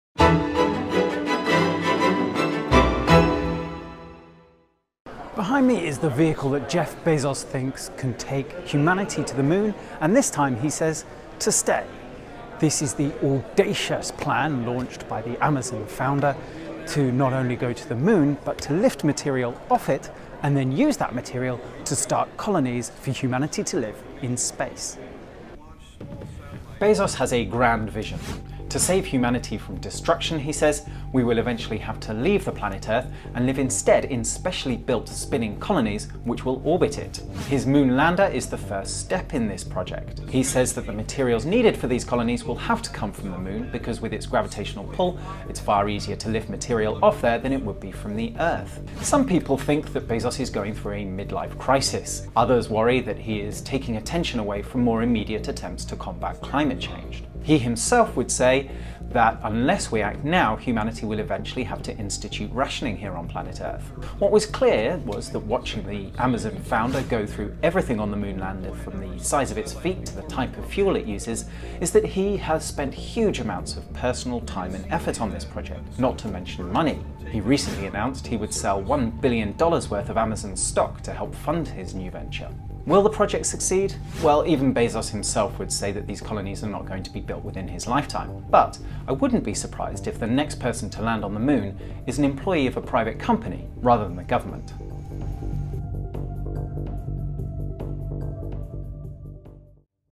Jeff Bezos sets his sights on the moon | Tech Wash
Jeff Bezos has unveiled a plan for a vehicle designed to land on the moon, in what he said was the first step to build colonies for humans in space. Kiran Stacey reports